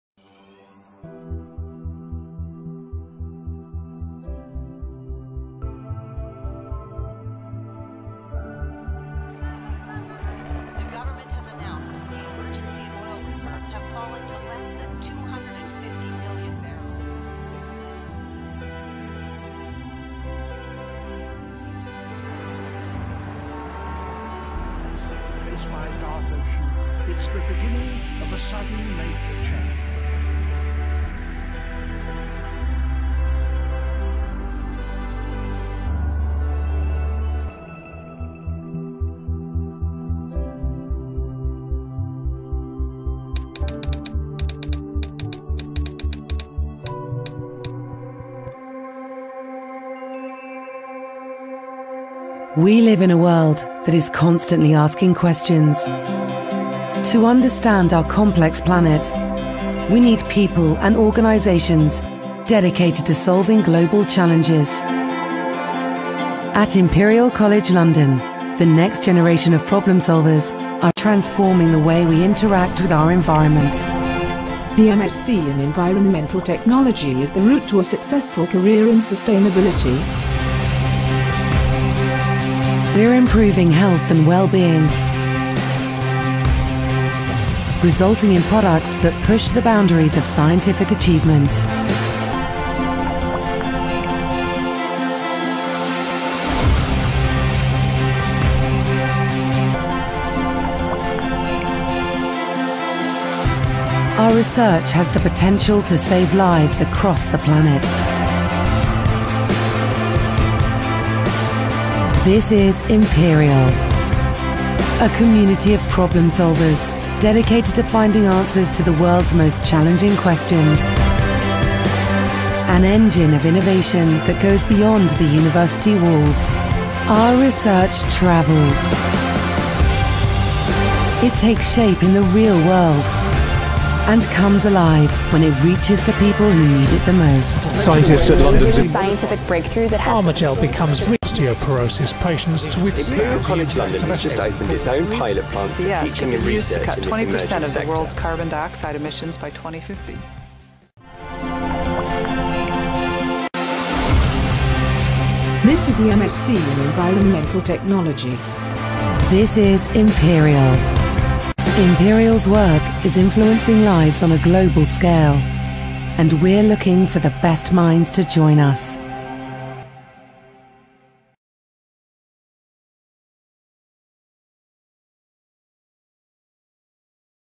MSc in Environmental Technology, Imperial College London
The MSc in Environmental Technology was established over three decades ago, and provides the highest standard of knowledge and skills development for environmental specialists.

The term technology in the course title refers to the application of scientific knowledge, policy and engineering to solve environmental problems and address sustainability.

You will acquire a diverse range of discipline-specific problem-solving frameworks for tackling contemporary environmental issues, while combining the natural and social sciences with engineering and medicine in a truly interdisciplinary manner.

A major emphasis of the course is on the way environments function and on the compatible tools, alternative technologies and policies for sustainable environmental management.

There are specialist options available in the second term, designed to cater for a variety of individual interests and career requirements.

You complete a research project which normally stems from one of the specialist options. The project term extends from April to September, with the whole of this period devoted to the project.

Your project may be laboratory or desk-based and can involve fieldwork. Projects are usually carried out in conjunction with outside organisations, and can take place in the UK or abroad.